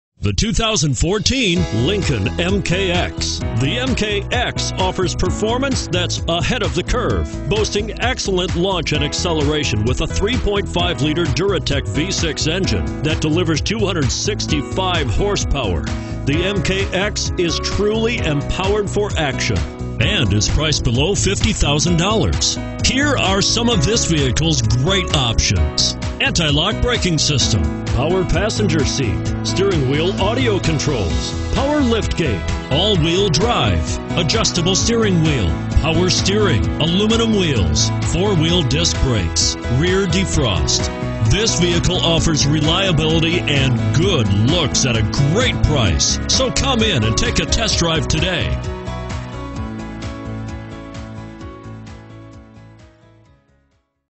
2014 LINCOLN MKX Egg Harbor, NJ MX14042
2014 LINCOLN MKX Egg Harbor, NJ
Stock# MX14042

Pick up this White Platinum Metallic Tri-Coat 2014 Lincoln MKX, available today. Featuring Automatic transmission, it has 0 miles. This could be the one you've been searching for.